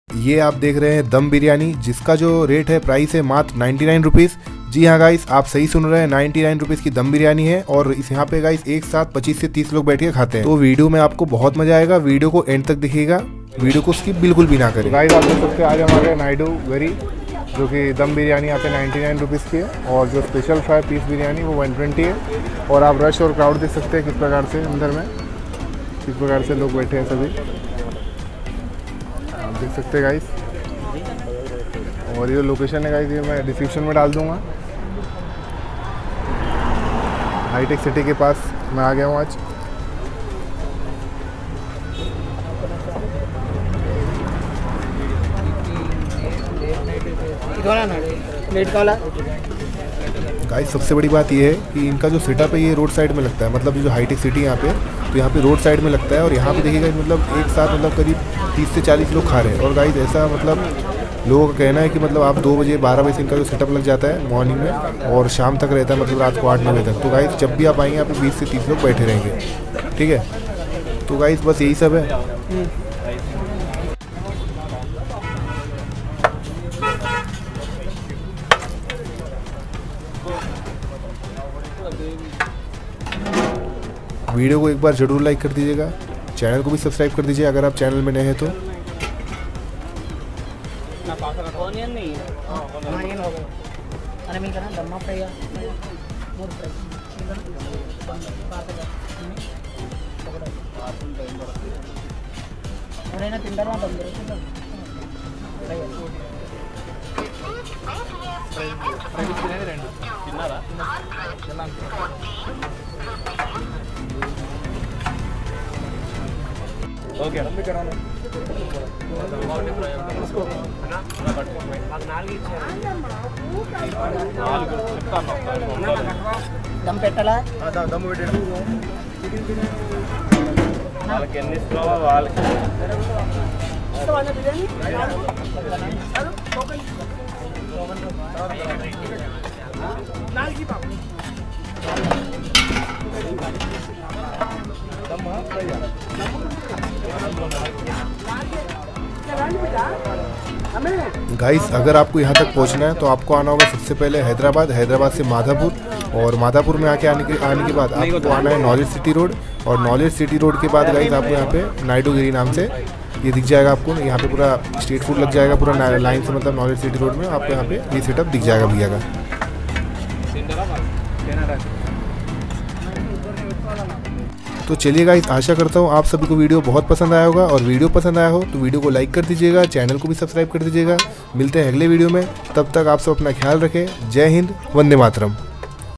Rush for Dum Biryani in Hyderabad 99 Rs/- Only | Indian Street Food
ADDRESS - NAIDU GIRI, KNOWLEDGE CITY ROAD, HIGH-TECH CITY, MADHAPUR, HYDERABAD, T.S., INDIA 🇮🇳

famous biryani of hyderabad
hyderabad
dum biryani
dum chicken biryani